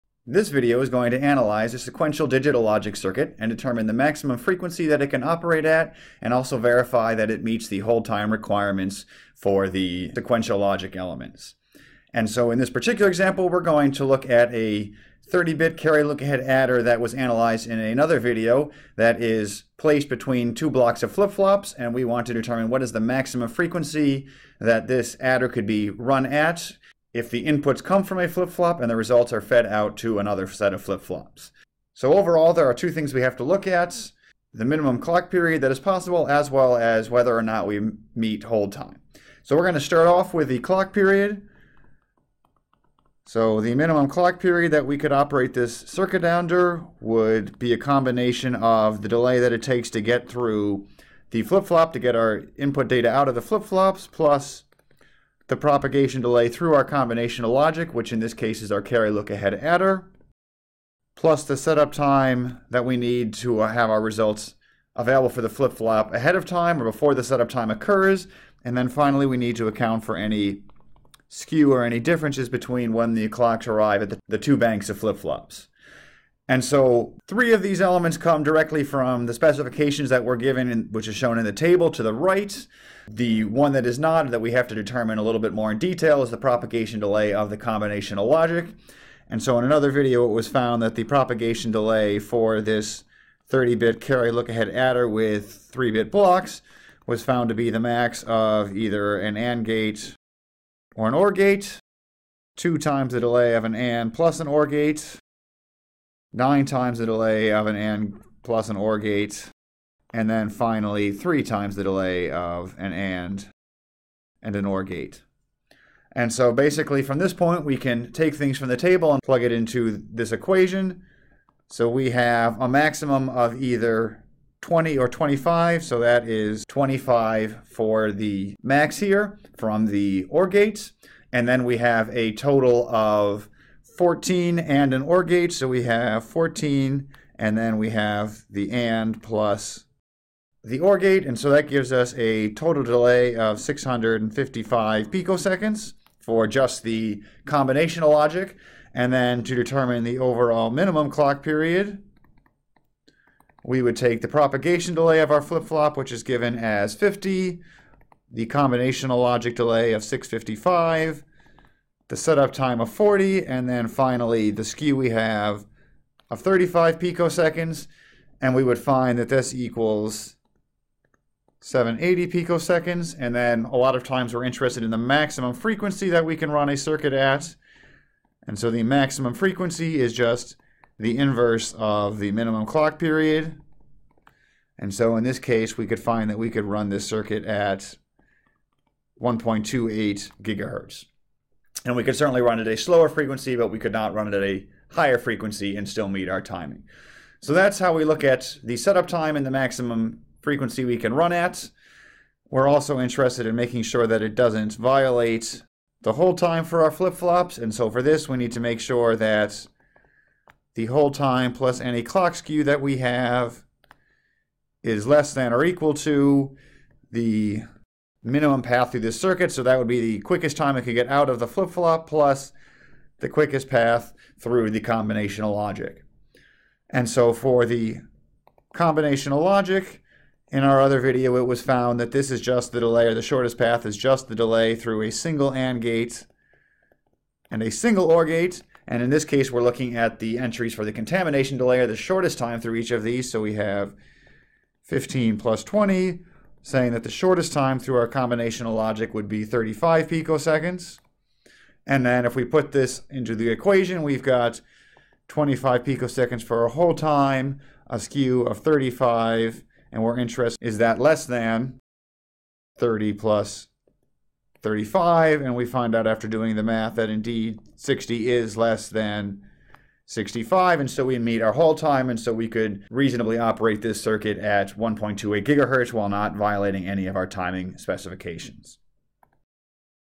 Sequential Logic Timing Example
Shows an example of determining the maximum frequency for a sequential logic circuit and verifying that it meets hold time.

for the development of the CLA combinational logic latency.